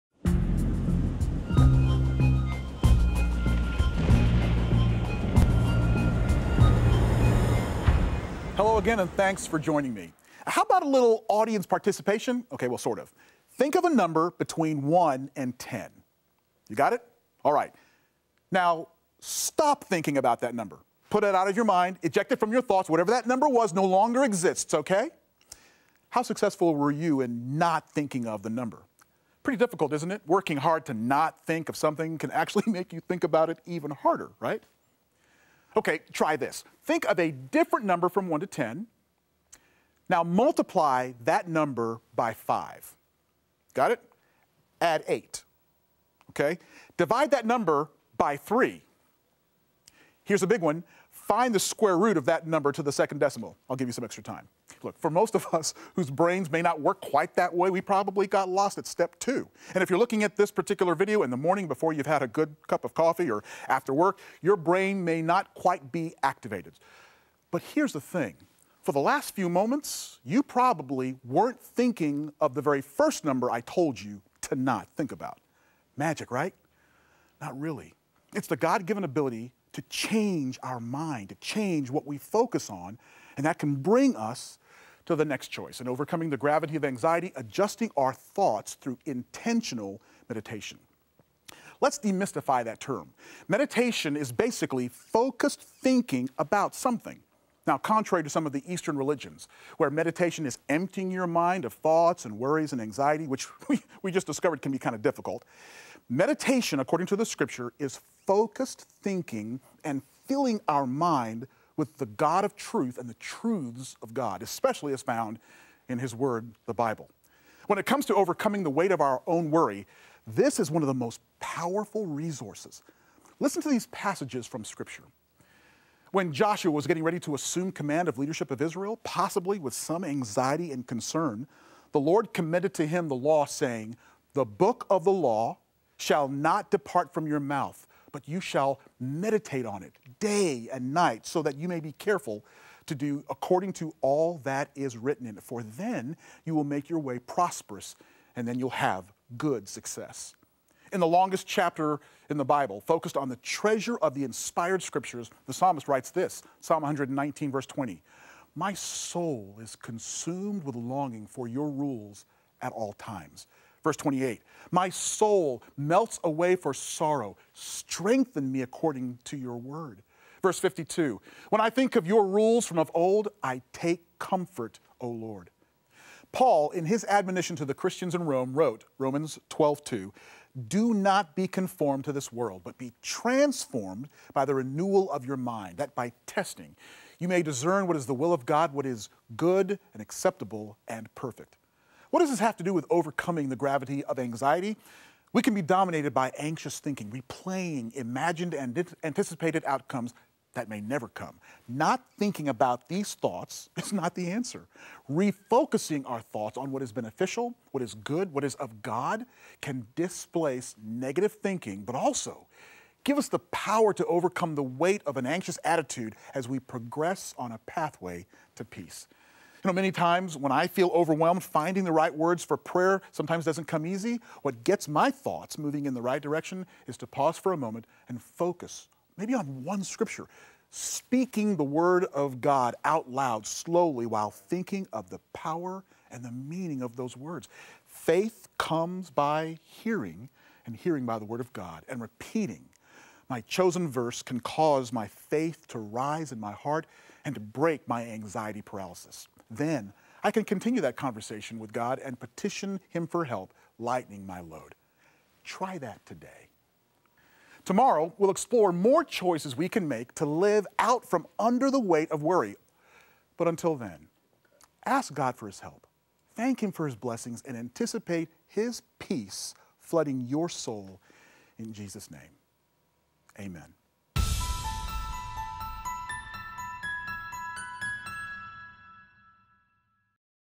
Strengthen Your Walk | Overcoming the Gravity of Anxiety | Adjust Your Thoughts Pt. 1 | Ep. 7
Ready to make a change in your thought patterns, so you can have more peace in your life?